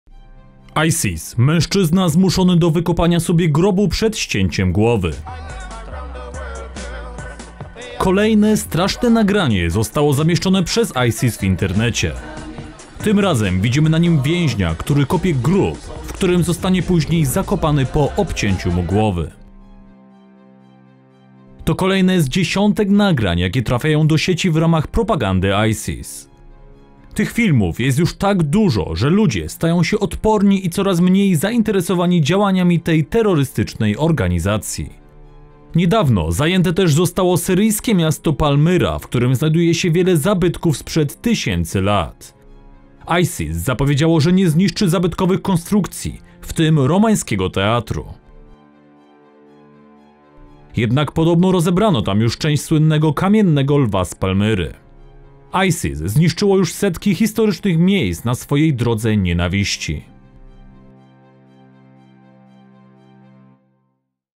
ISIS: mężczyzna zmuszony do wykopania sobie grobu przed ścięciem głowy
Kolejne straszne nagranie zostało zamieszczone przez ISIS w internecie.
Tym razem widzimy na nim więźnia, który kopie grób, w którym zostanie później zakopany po obcięciu mu głowy.
To kolejne z dziesiątek nagrań jakie trafiają do sieci w ramach propagandy ISIS.
Nagrań jest już tak dużo, że ludzie stają się odporni i coraz mniej zainteresowani działaniami tej terrorystycznej organizacji.
Niedawno zajęte też zostało Syryjskie miasto Palmyra, w którym znajduje się wiele zabytków sprzed tysięcy lat.
ISIS zapowiedziało, że nie zniszczy zabytkowych konstrukcji, w tym romańskiego teatru.
Jednak podobno rozebrano już na części słynnego kamiennego lwa z Palmyry.
ISIS zniszczyło juz setki historycznych miejsc na swojej drodze nienawiści.